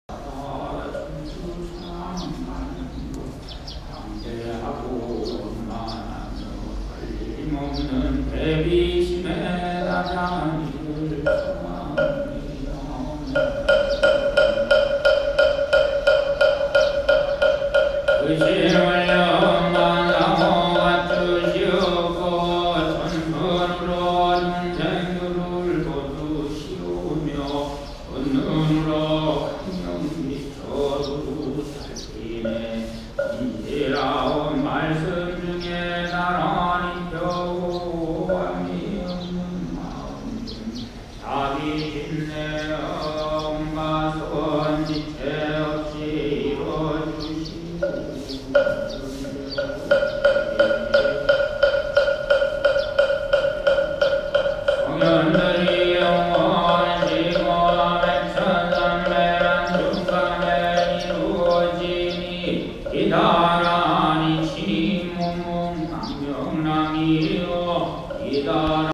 Beginning our South Korean Tea Tour May 2016 - Yogyesa Temple, Seoul.
Yogjesa Temple, Seoul. Where we will be beginning our South Korean Tea Tour in May 2016.